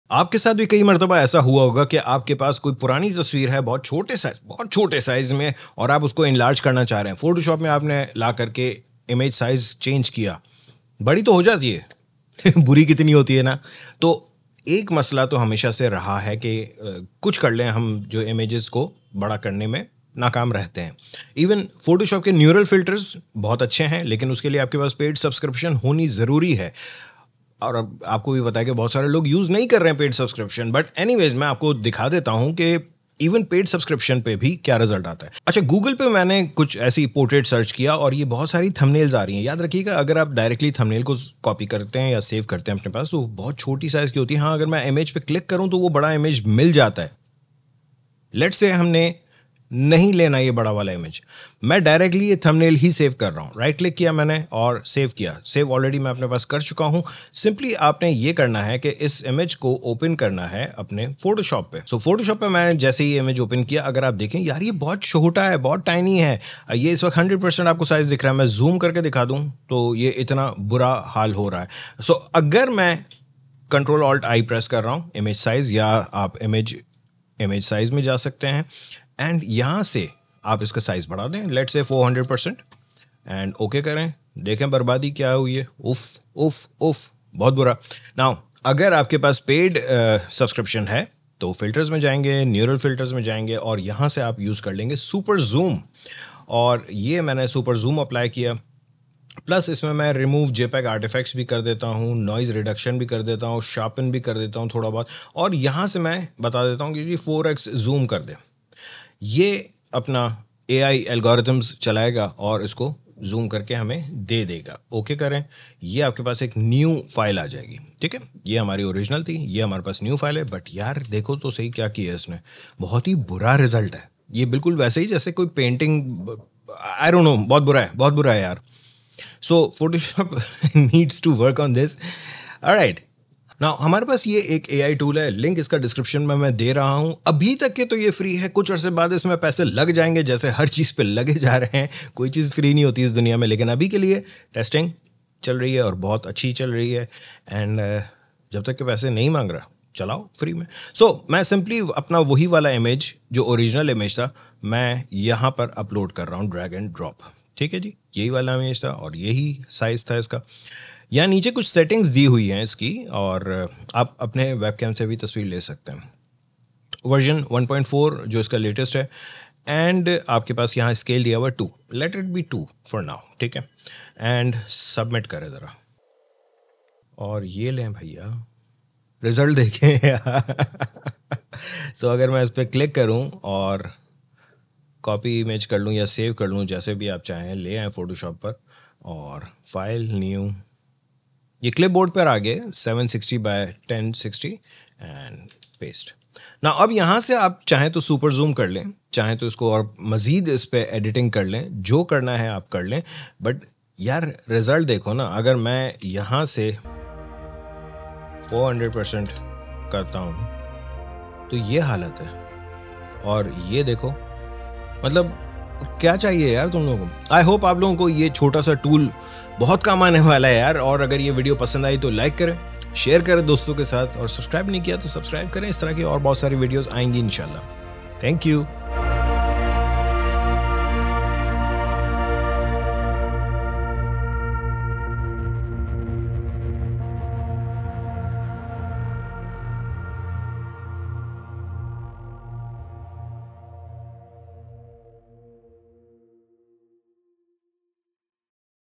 Resize Images with Artificial intelligence AI - اردو / हिंदी [Eng Sub]
Use Artificial intelligence (AI) to resize your images with perfection. It was nearly impossible to resize images with full details now with this tool you can restore old pixelated photos of your loving memories.